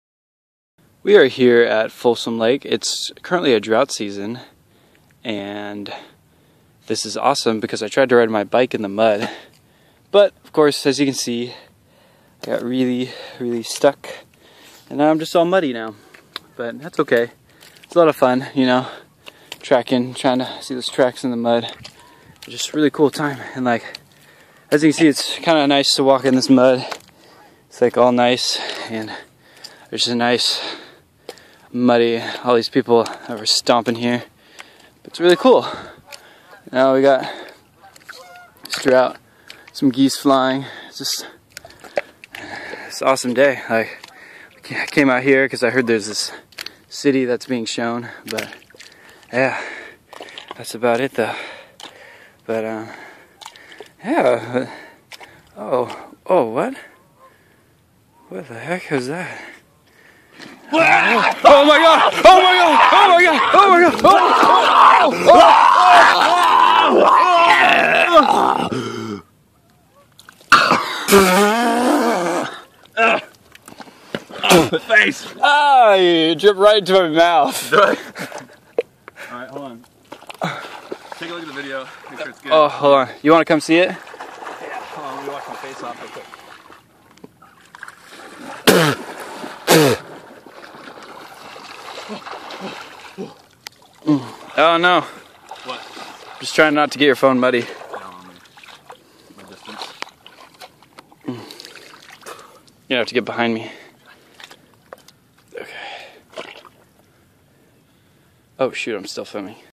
Animal Encounters: Biker attacked at Folsom Lake
He finds himself stuck in mud when he is attacked.